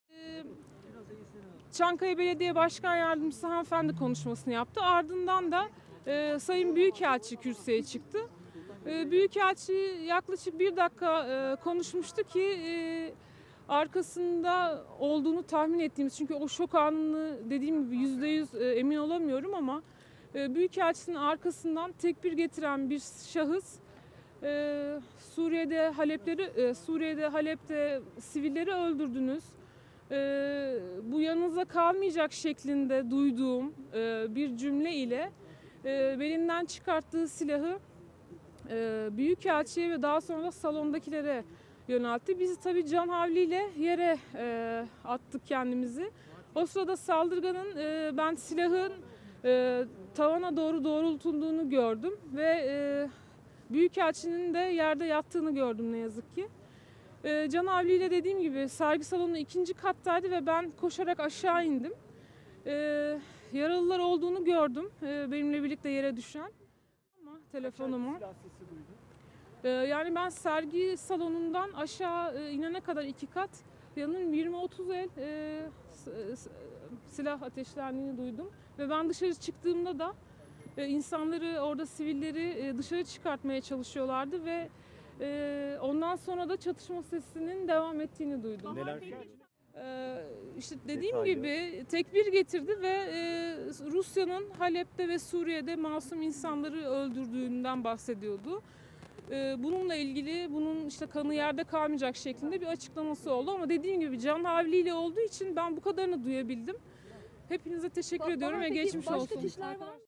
Büyükelçiye yapılan suikast sonrası görgü tanığı açıklaması
Rus Büyükelçi Karlov'a yapılan saldırının görgü tanığı konuştu.